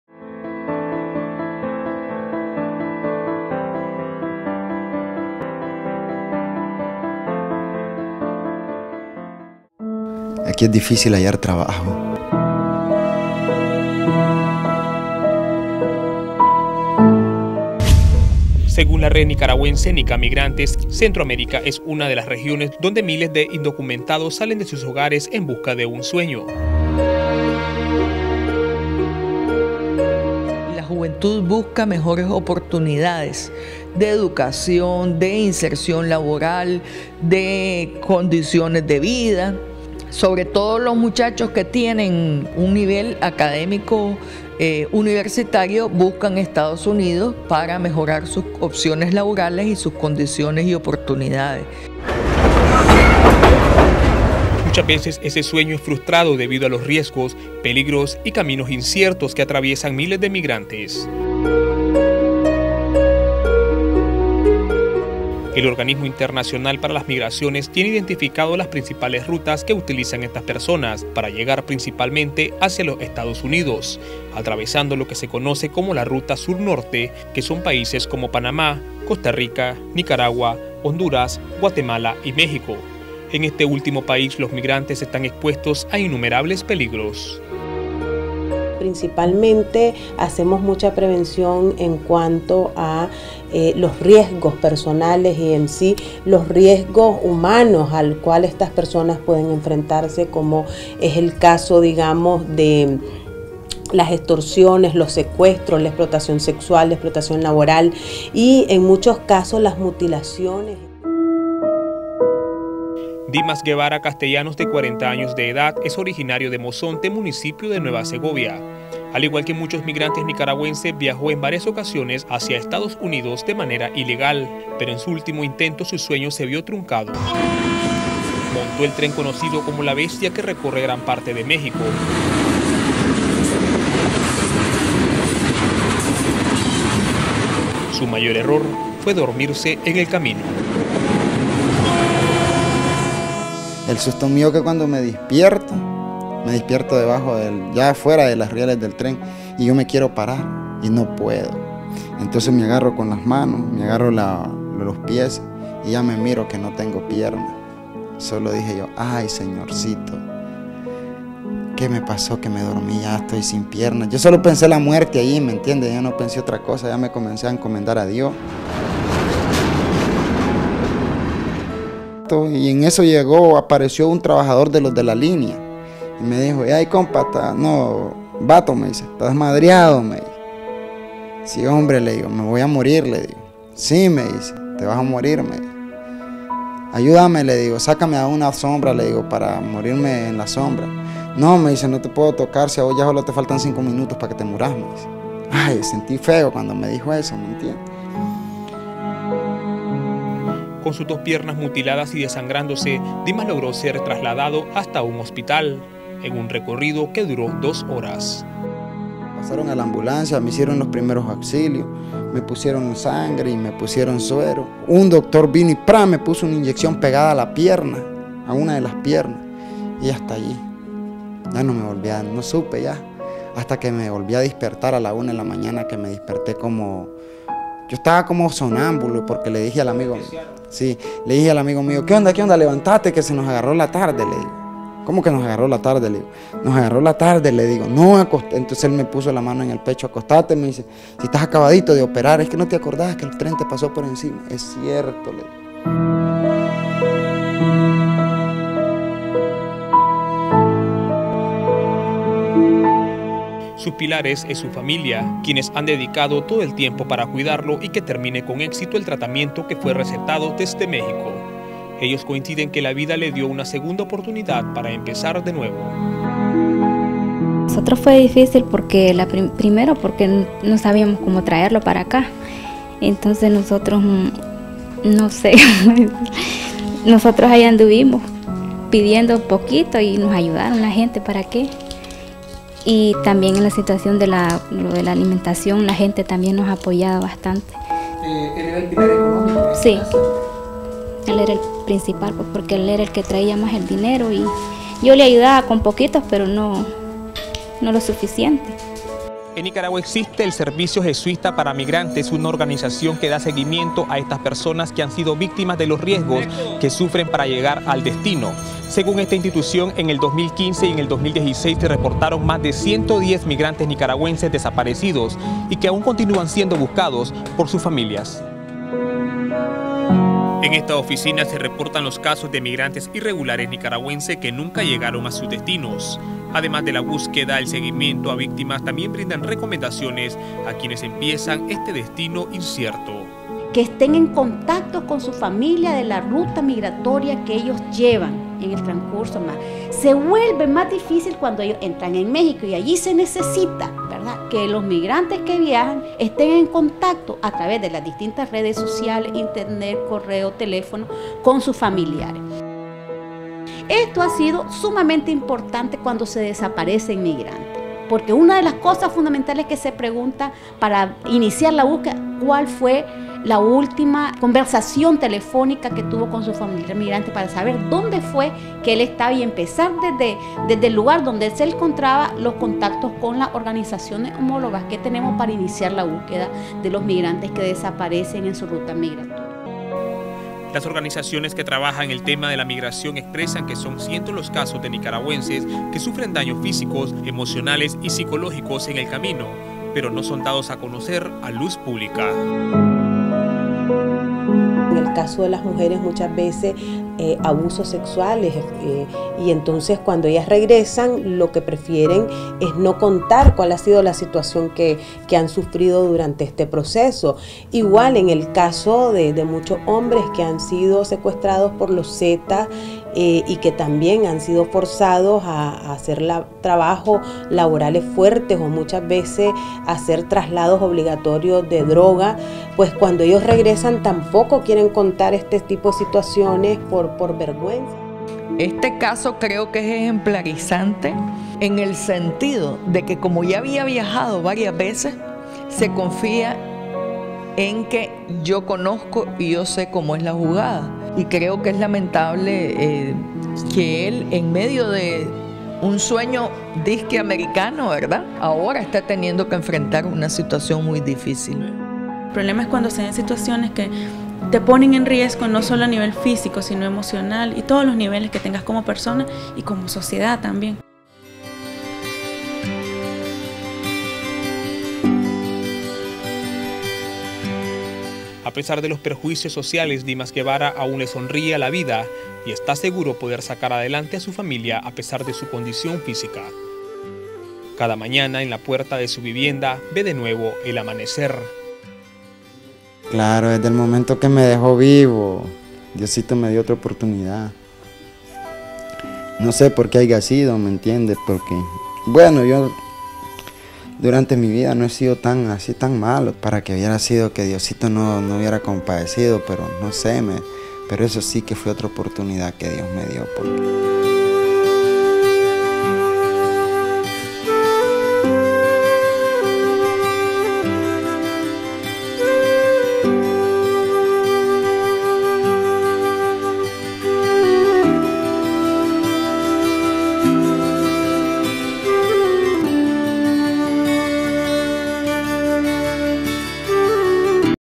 Notivos: Nicaragüense pierde sus piernas en busca del sueño americano
Migración un destino incierto es un reportaje que aborda la historia del nicaragüense que perdió sus piernas en busca del sueño americano.